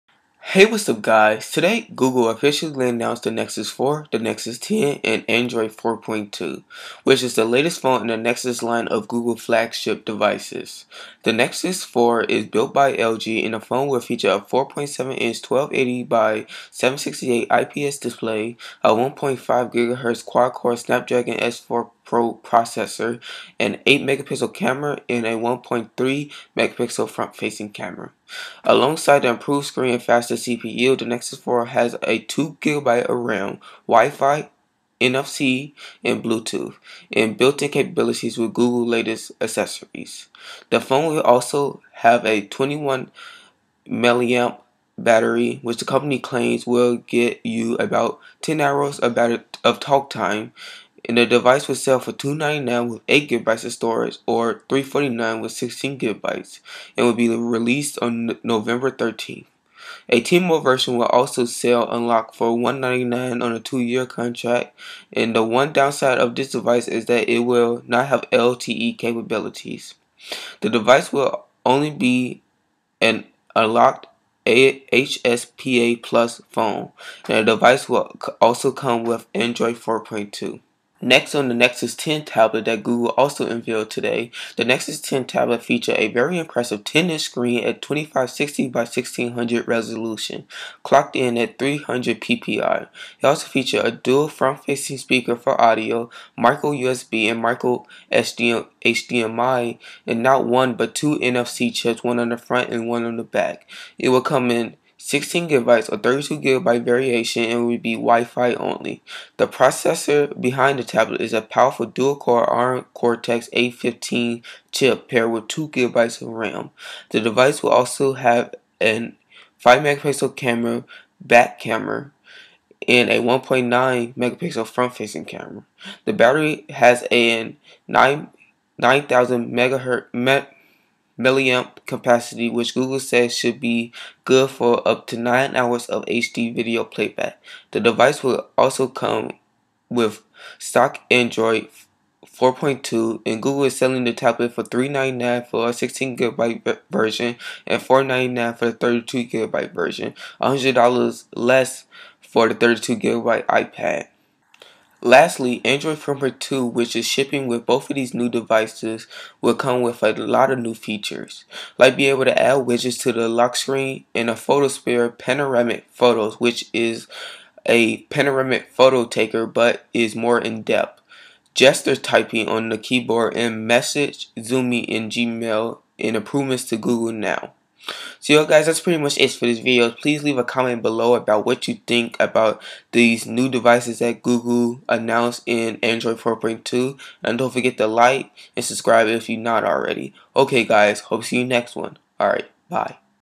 Nexus 4, Nexus 10 & Android 4.2 Overview
Today I give a overview of  Google officially announced the Nexus 4 phone, Nexus 10 tablet and Android 4.2.